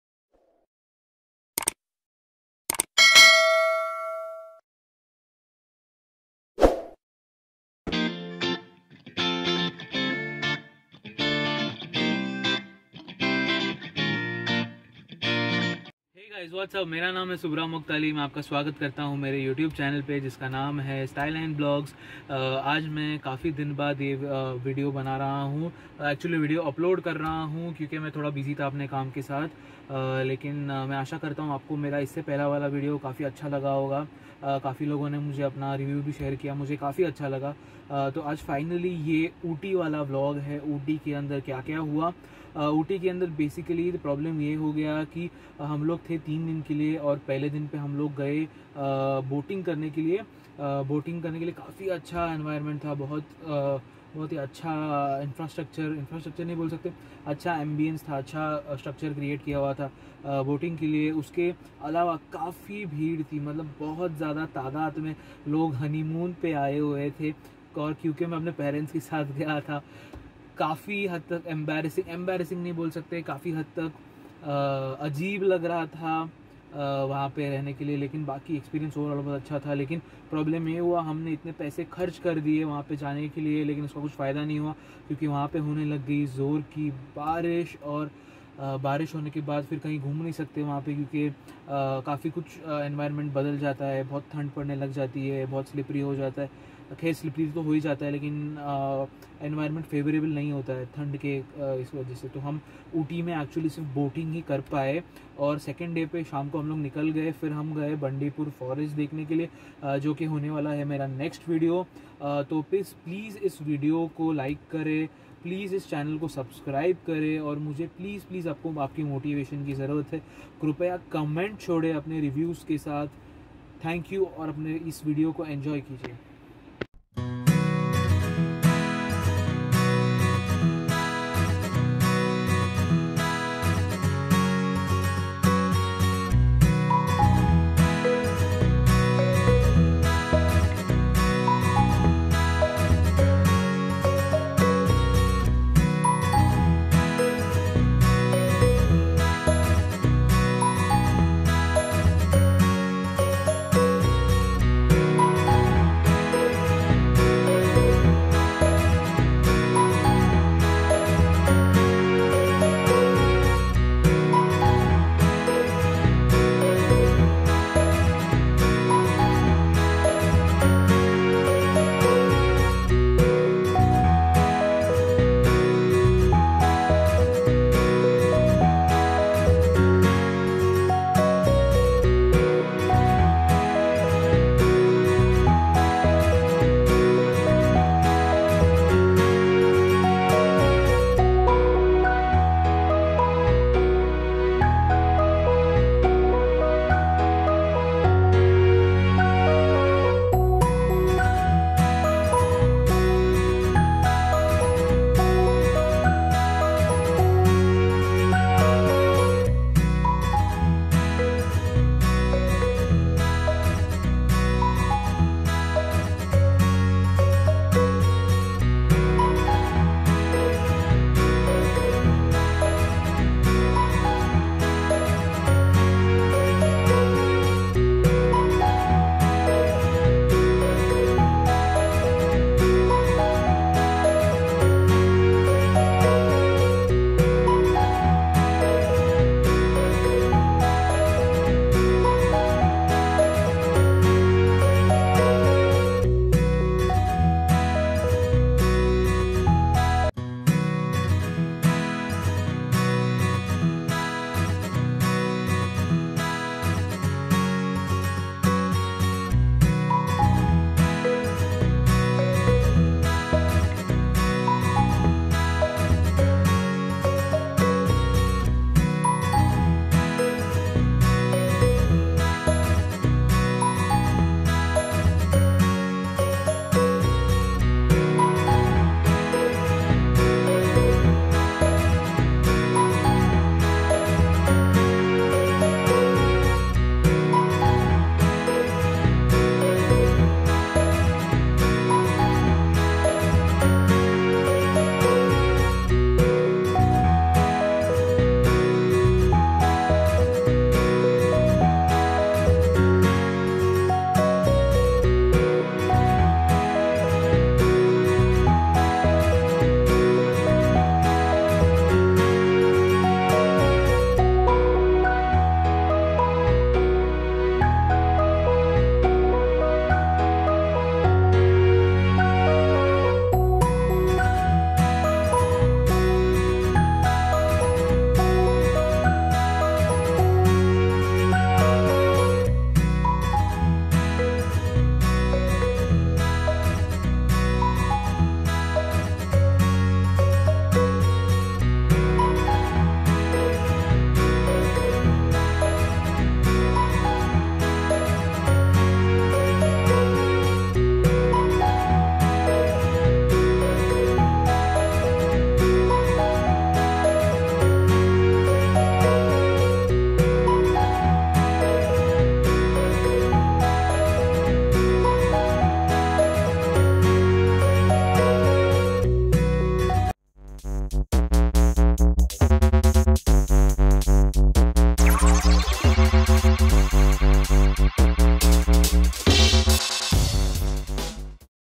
Boat House | Ooty vlog 2 | It started raining and trip got cancelled
Hey there guys ! Thank you for showing constant love , please to leave your valuable review in the comment section
Talking about this video , we visited ooty and this is the part 2 of the trip